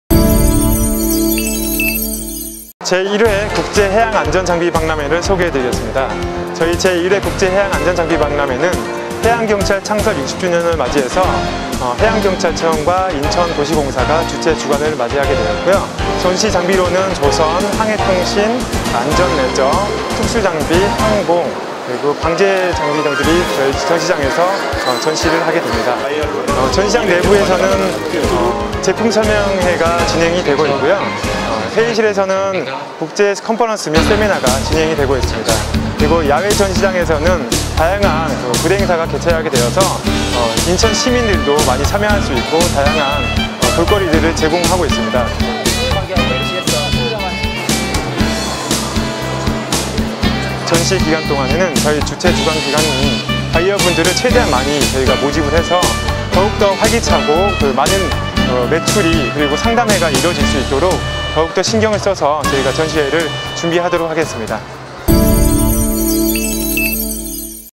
[영상] '제1회 국제 해양안전장비박람회' 현장 스케치
인천도시공사(사장 유영성)와 해양경찰청(청장 김석균)은 공동 주최로 '제1회 국제 해양안전장비박람회'를 오는 9월 11일부터 13일까지 3일간 인천 송도컨벤시아에서 개최한다.